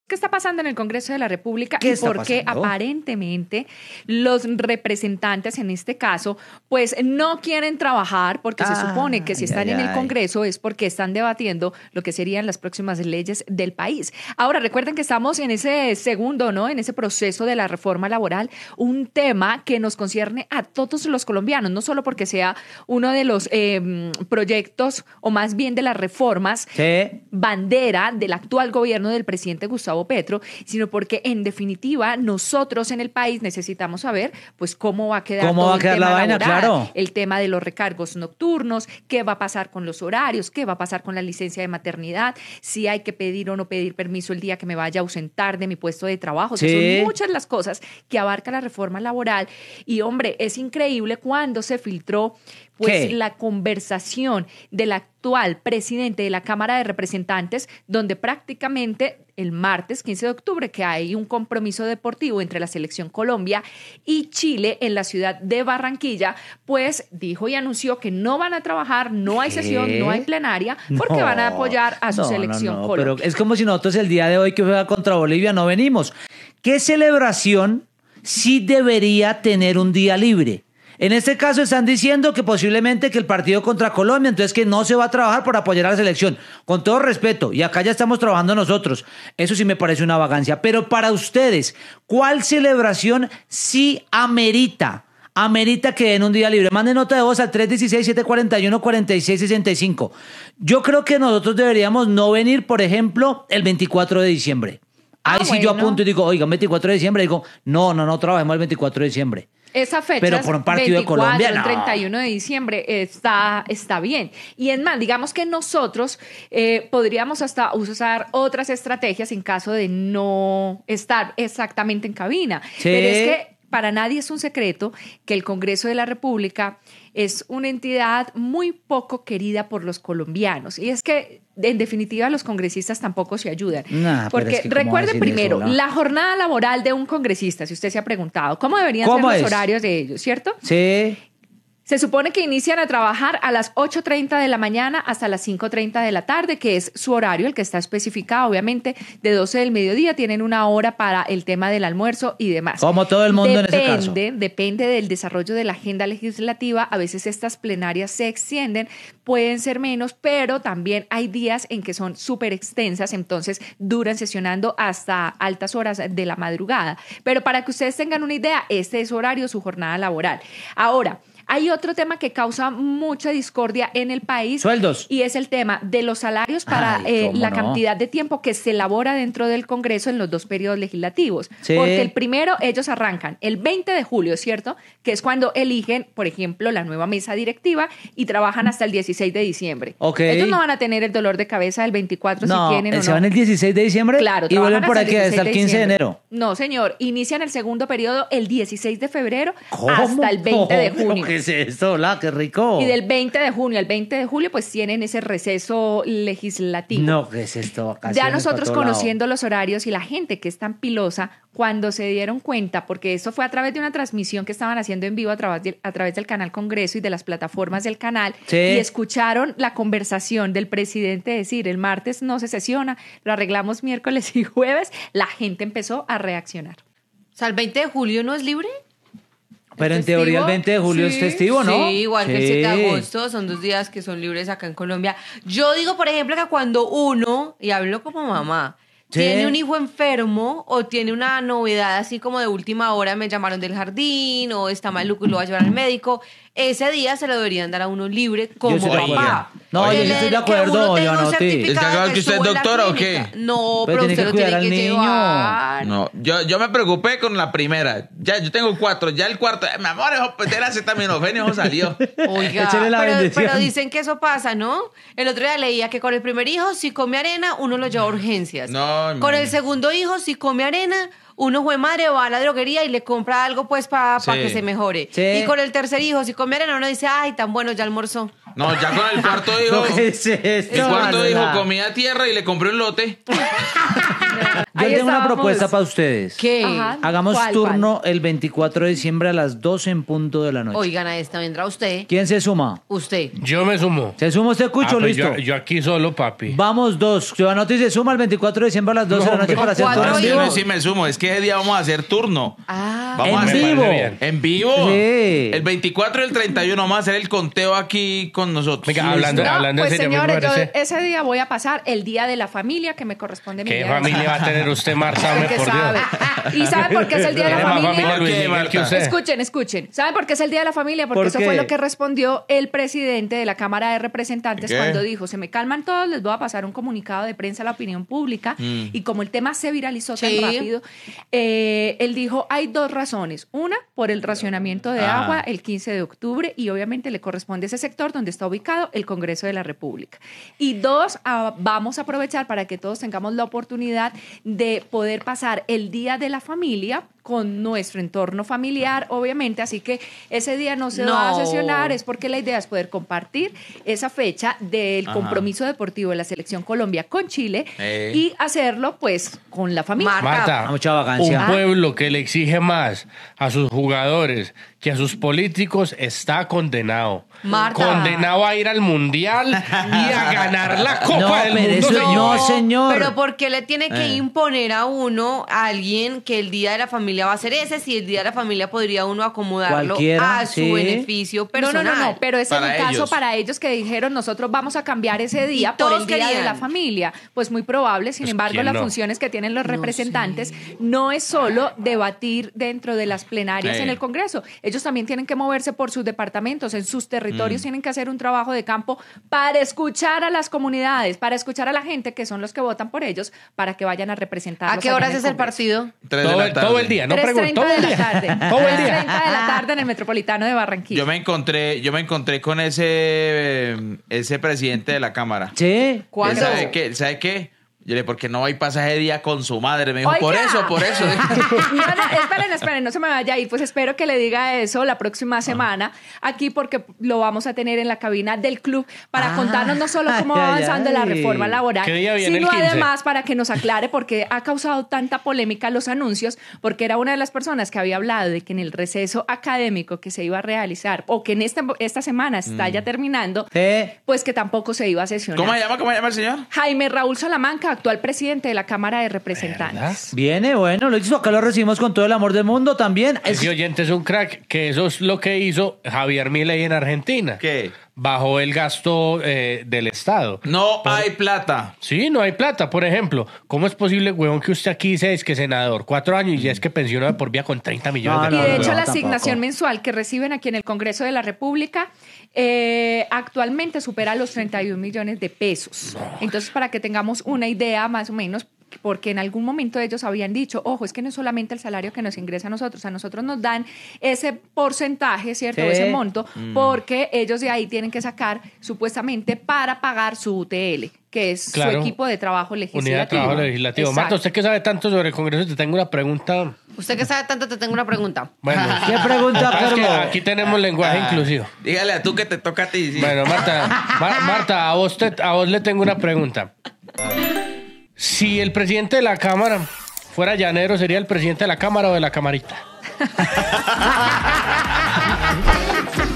Polémica por congresistas que no van a trabajar el día que va a jugar la Selección Colombia
Una gran controversia se generó en las redes, luego de que el presidente de la Cámara de Representantes revelara que el 15 de octubre, justo el día que juega la Selección Colombia en Barranquilla, los congresistas no van a trabajar.